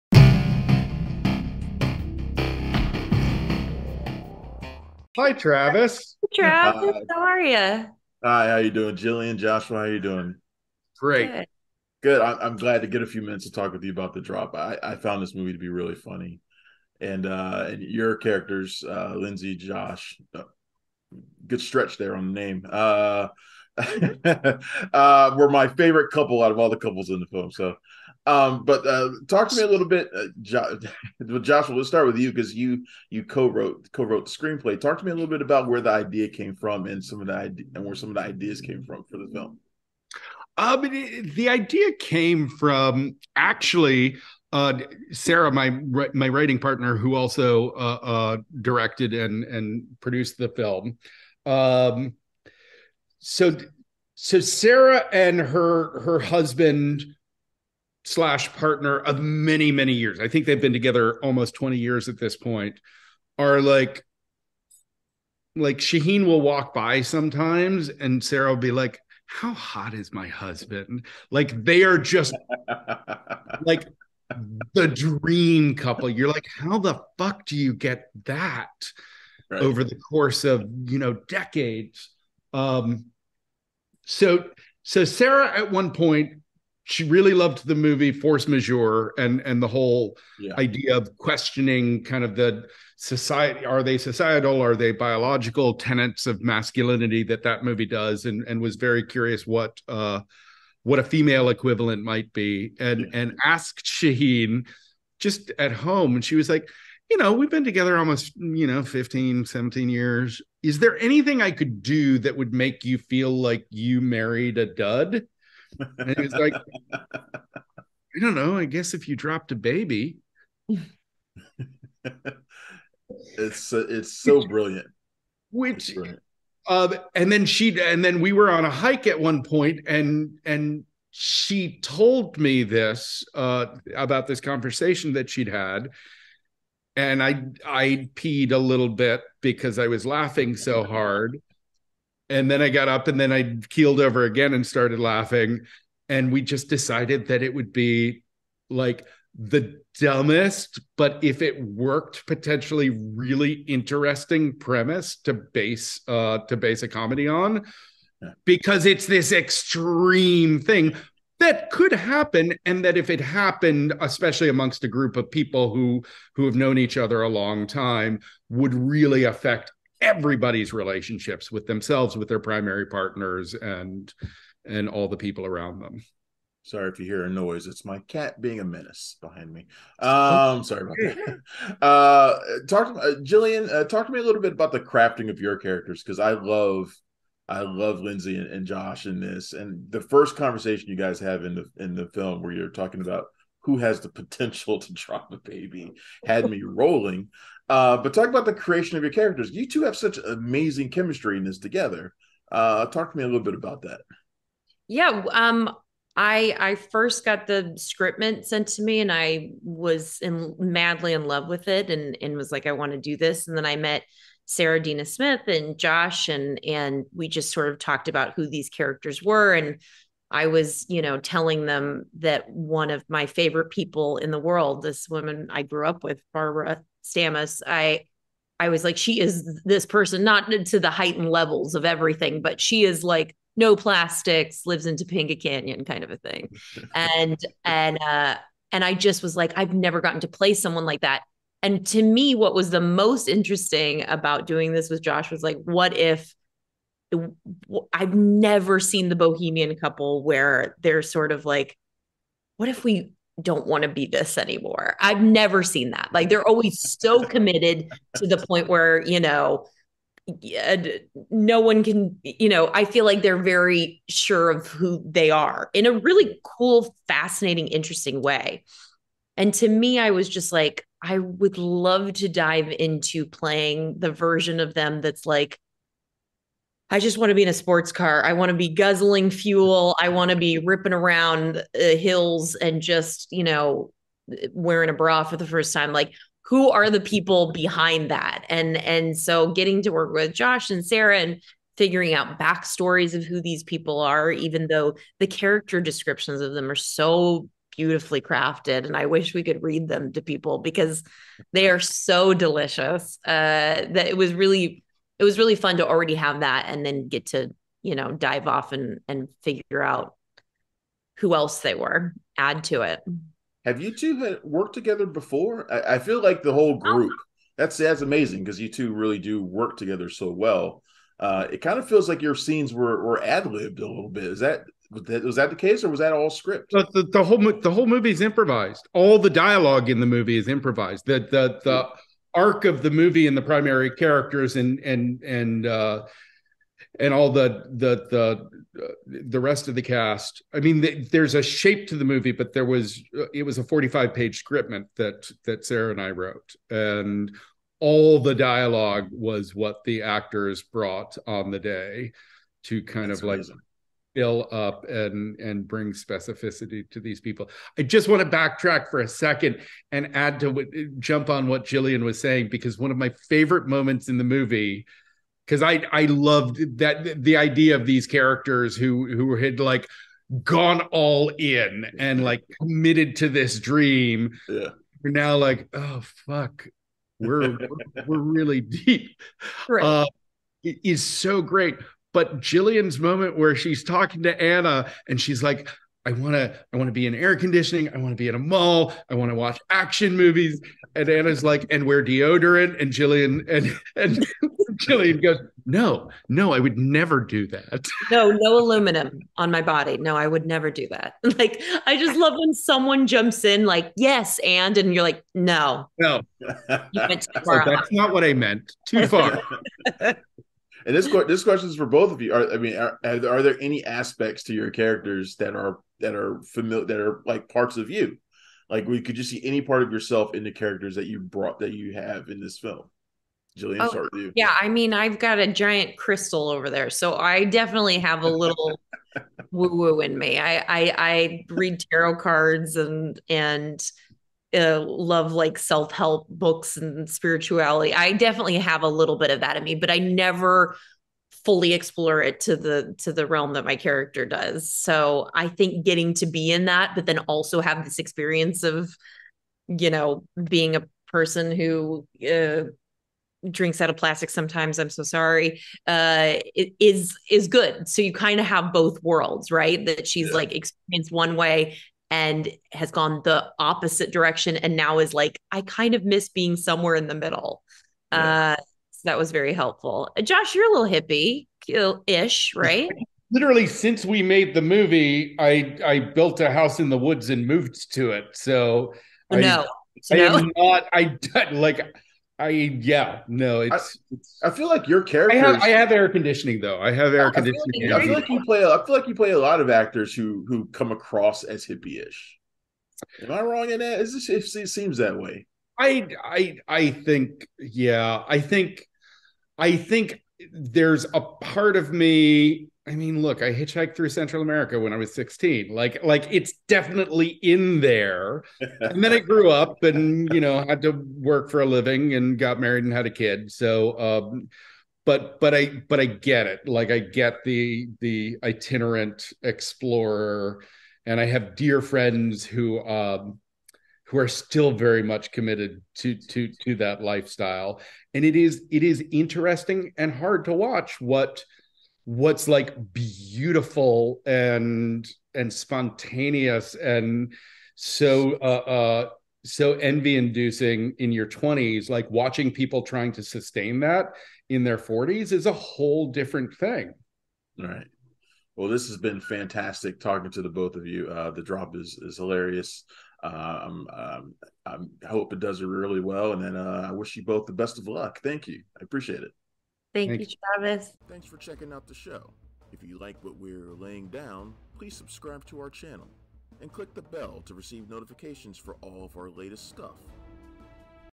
Jillian Bell And Joshua Leonard Talk Crazy Cringe Comedy THE DROP
Jillian Bell and Joshua Leonard join Travis Hopson of Punch Drunk Critics to talk about their new Hulu comedy THE DROP. An awkward comedy of manners, the film follows the aftermath of a baby being dropped on its head at a Destination Wedding getaway.